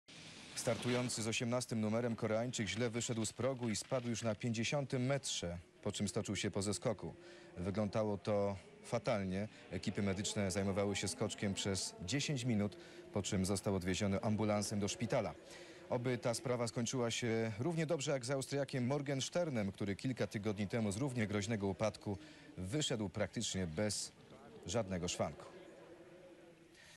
Heung Chul Choi - HORRIBLE CRASH - Engelberg 2003 (TVN)
Jako że dziś mamy pierwszy w historii konkurs transmitowany w całości na TVN, to zdecydowałem się na puszczenie takiego smaczka sprzed kilkunastu lat :p